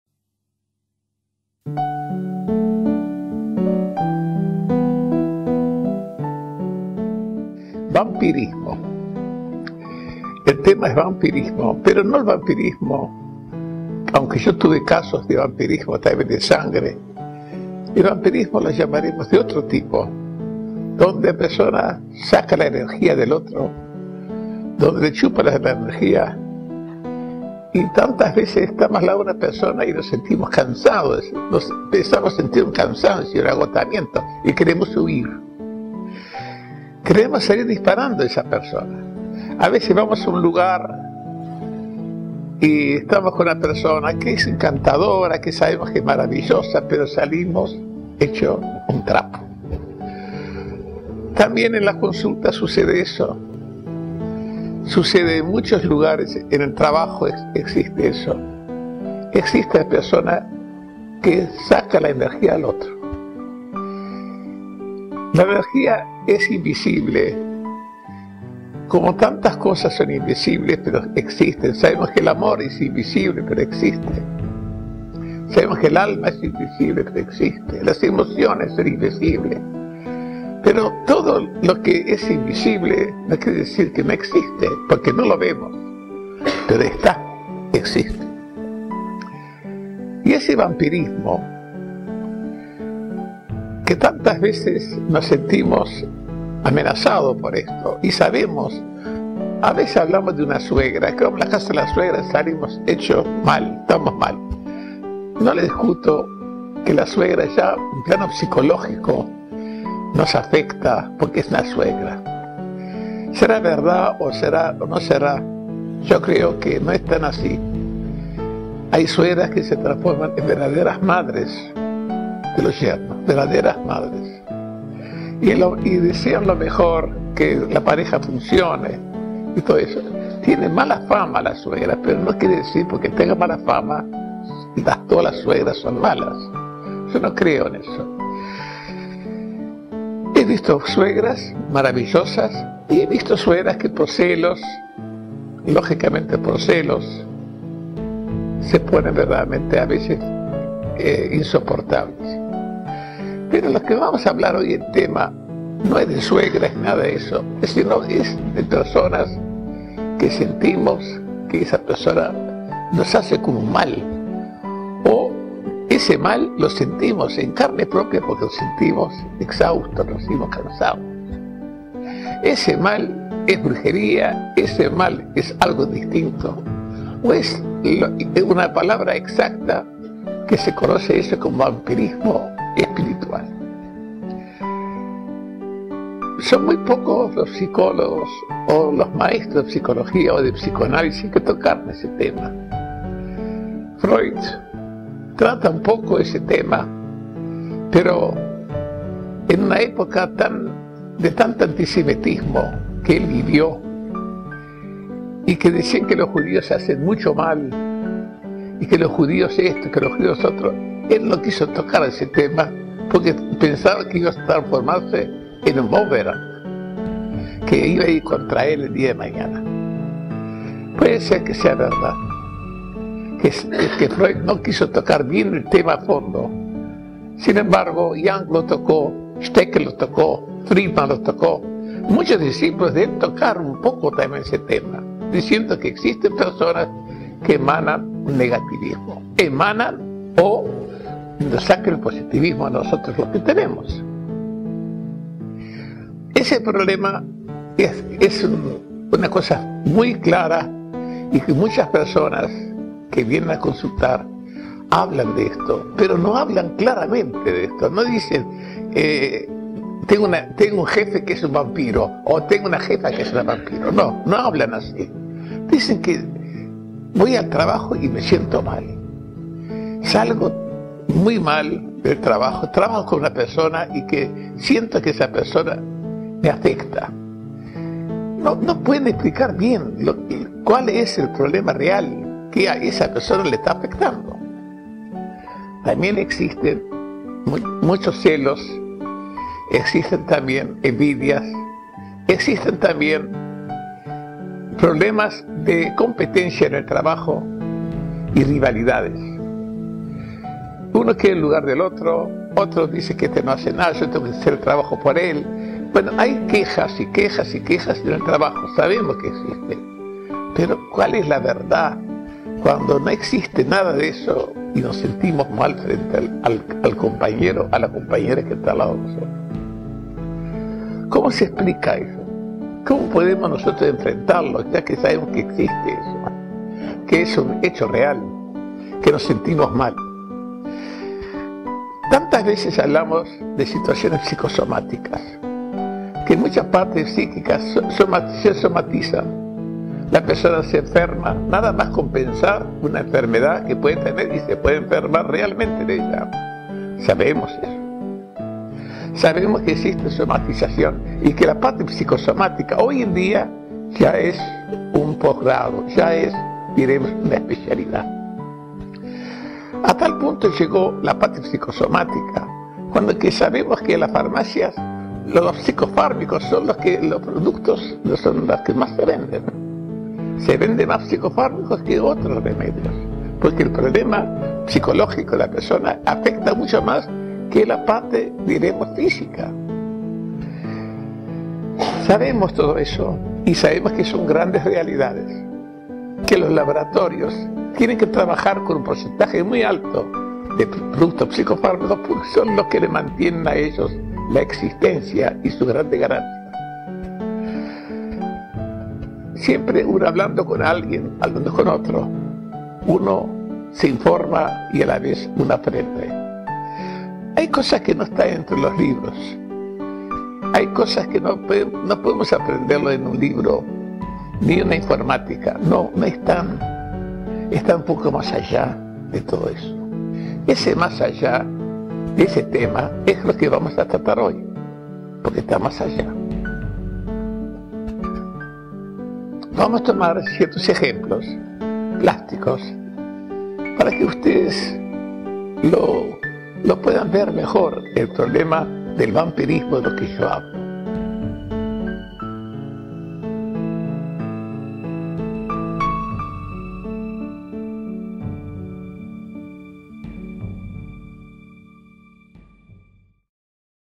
Vampirismo en el Aura
"Cómo protegerse del Vampirismo".Cuando un Aura se alimenta energéticamente de otra, se puede manifestar con síntomas físicos (cefaleas, malestar estomacal), pérdida del ánimo, apatía, mal humor ..y hasta "mala suerte".
El Kabash te enseña a combatir el negativismo del Aura y a desarrollar la energía positiva para alcanzar el éxito.